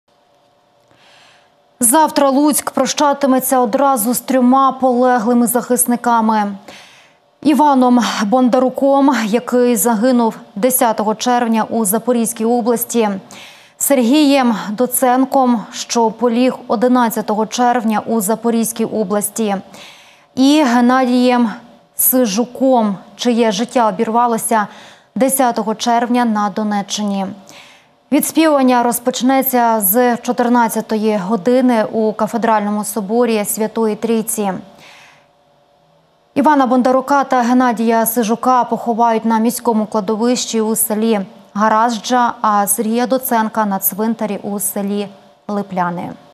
Громада Луцька проведе в останню дорогу трьох захисників
Завтра Луцьк прощатиметься одразу з трьома полеглими захисниками: Іваном Бондаруком, який загинув 10 червня у Запорізькій області, Сергієм Доценком, що поліг 11 червня  у Запорізькій області і Геннадієм Сижуком, чиє життя обірвалось 10 червня на Донеччині. Відспівування розпочнеться з 14 години у Кафедральному соборі Святої Трійці.  Івана Бондарука та Геннадія Сижука поховають на міському кладовищі у селі Гаразджа, а Сергія Доценка – на цвинтарі у селі Липляни.